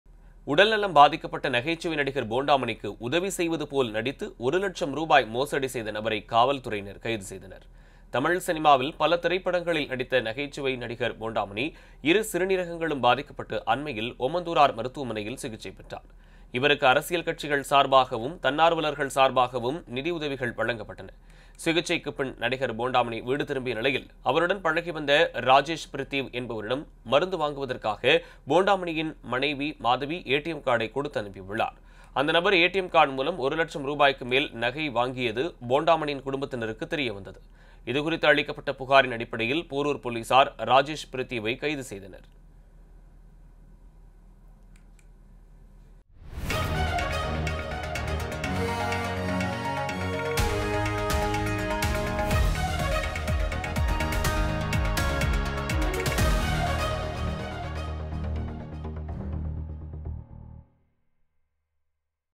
நடிகர் போண்டாமணியிடம் ரூ.1 லட்சம் ஏமாற்றியவர் கைது:ஏடிஎம் கார்டில் நகை வாங்கி மோசடி
நடிகர் போண்டாமணியிடம் ரூ.1 லட்சம் ஏமாற்றியவர் கைது:
ஏடிஎம் கார்டில் நகை வாங்கி மோசடி